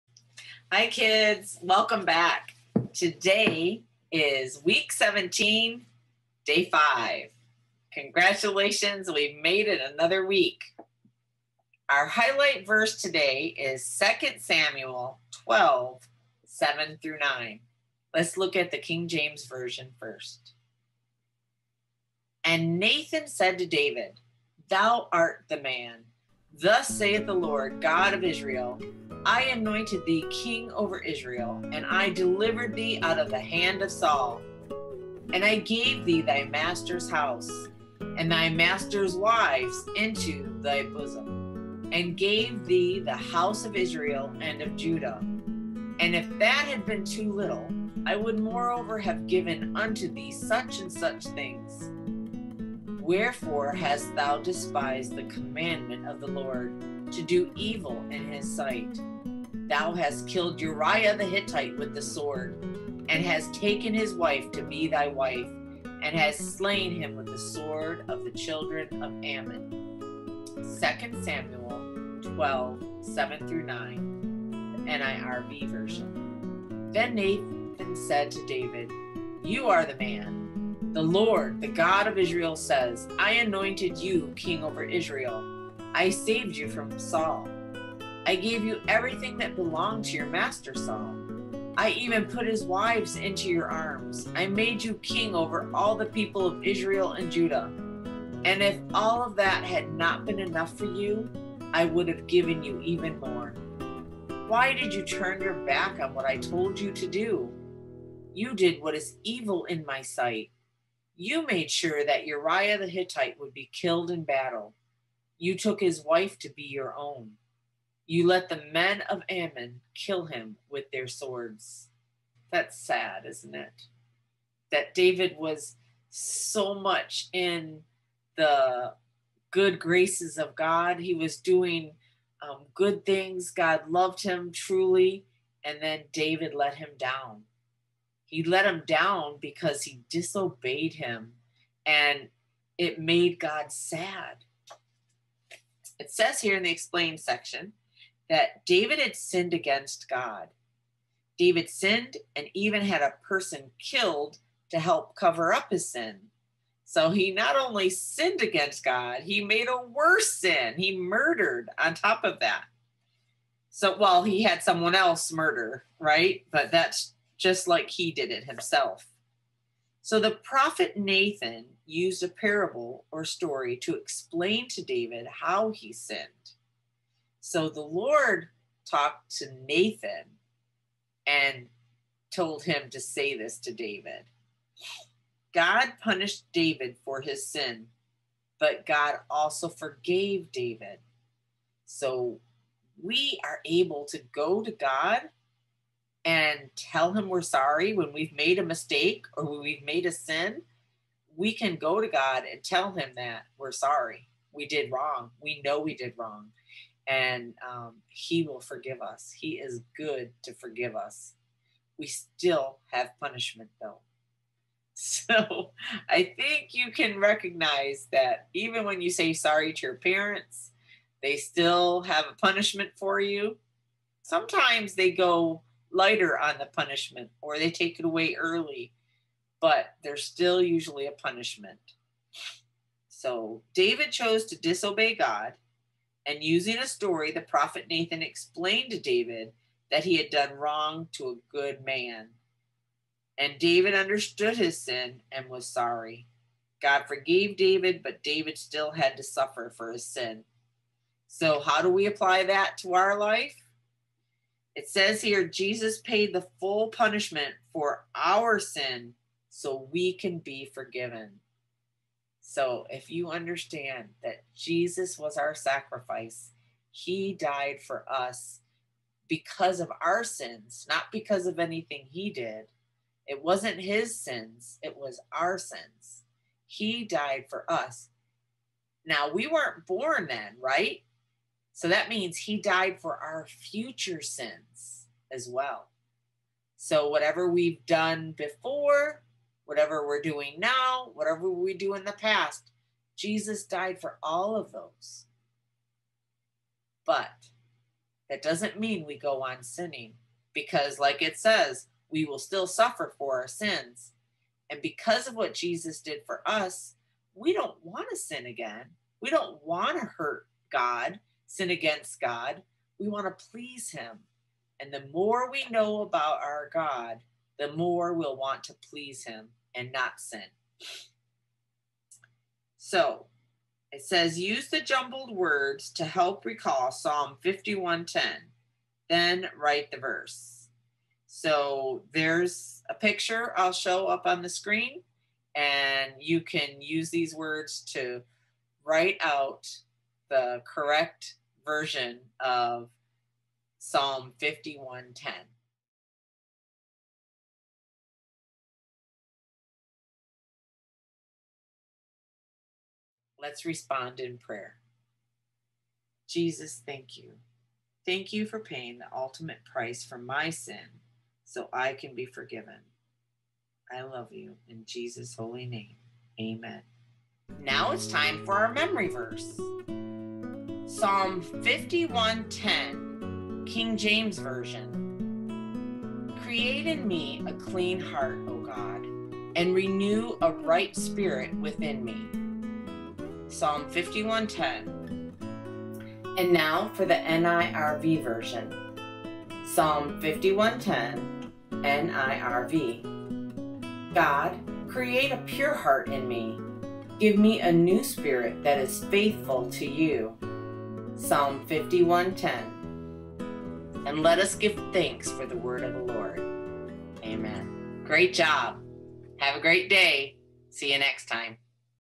2 Samuel 12:7-9 Kids Daily Reading Plan Week 17 Day 5
2 Samuel 12:7-9 Kids Daily Reading Plan

2 Samuel 12:7-9 KJV
7 And Nathan said to David, Thou art the man. Thus saith the Lord God of Israel, I anointed thee king over Israel, and I delivered thee out of the hand of Saul;
8 And I gave thee thy master's house, and thy master's wives into thy bosom, and gave thee the house of Israel and of Judah; and if that had been too little, I would moreover have given unto thee such and such things.
9 Wherefore hast thou despised the commandment of the Lord, to do evil in his sight? thou hast killed Uriah the Hittite with the sword, and hast taken his wife to be thy wife, and hast slain him with the sword of the children of Ammon.

2 Samuel 12:7-9 NIRV
7 Then Nathan said to David, “You are the man! The Lord, the God of Israel, says, ‘I anointed you king over Israel. I saved you from Saul. 8 I gave you everything that belonged to your master Saul. I even put his wives into your arms. I made you king over all the people of Israel and Judah. And if all of that had not been enough for you, I would have given you even more. 9 Why did you turn your back on what I told you to do? You did what is evil in my sight. You made sure that Uriah, the Hittite, would be killed in battle. You took his wife to be your own. You let the men of Ammon kill him with their swords.

Week 17  Memory Verse Psalm 51:10
Create in me a clean heart, O God; and renew a right spirit within me.

Psalm 51:10 NIRV
God, create a pure heart in me.
Give me a new spirit that is faithful to you.